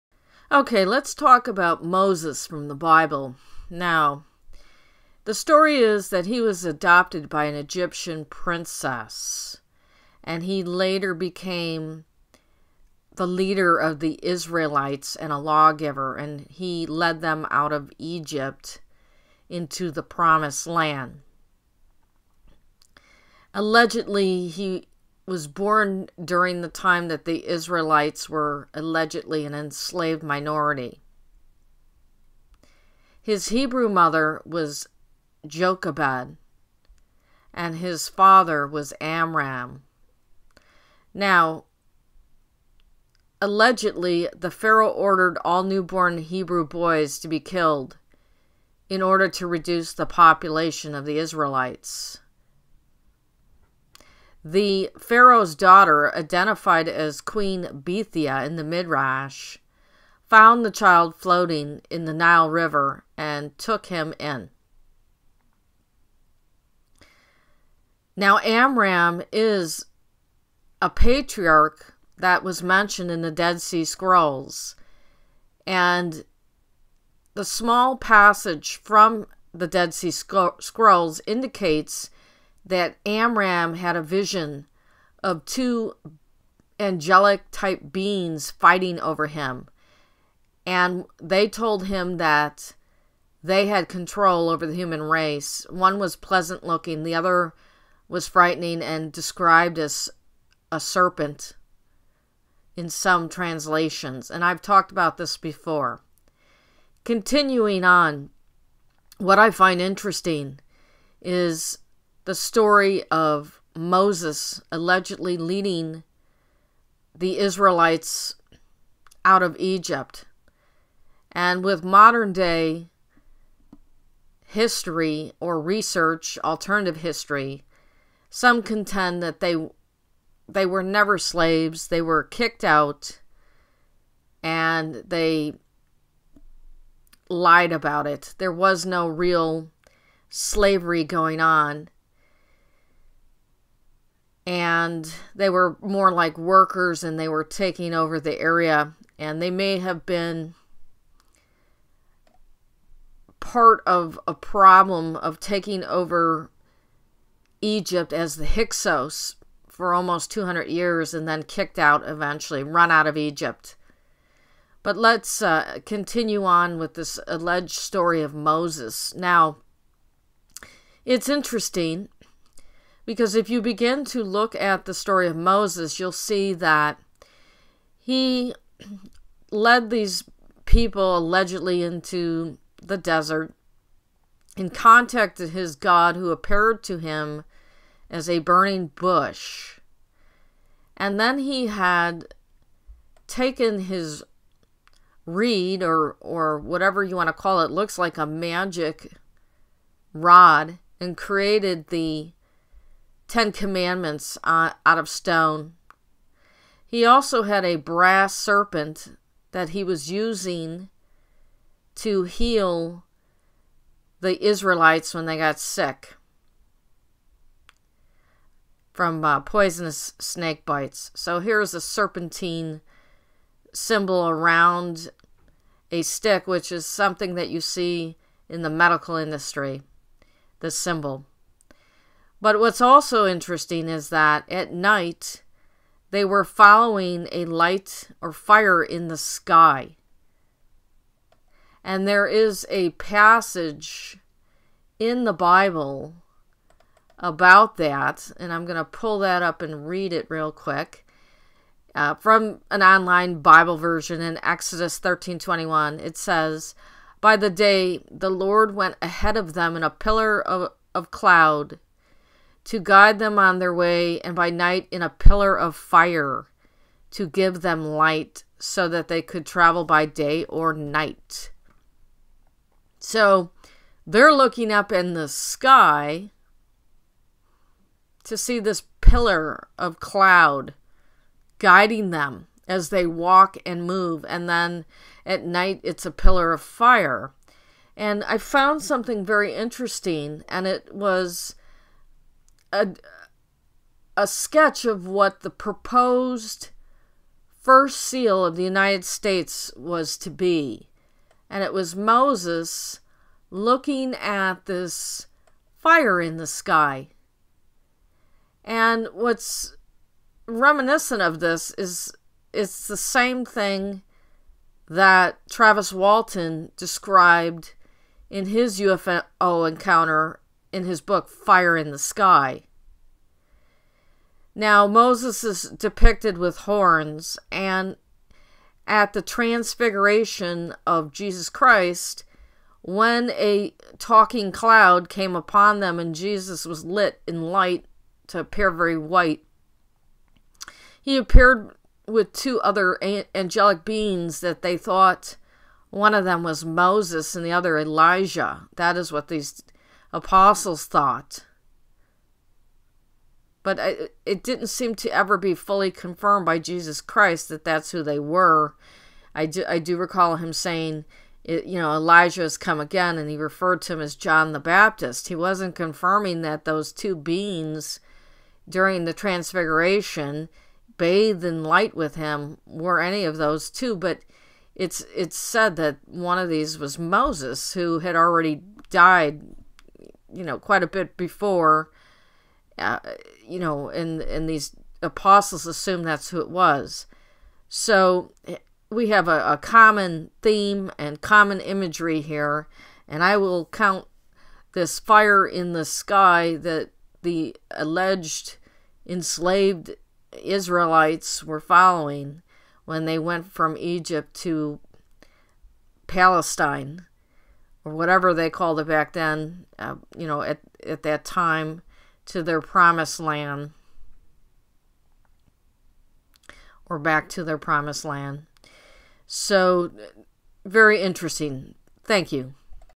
UFO/Aliens #1
Moses had multiple UFO encounters.  The Bible is full of them.  What is the truth?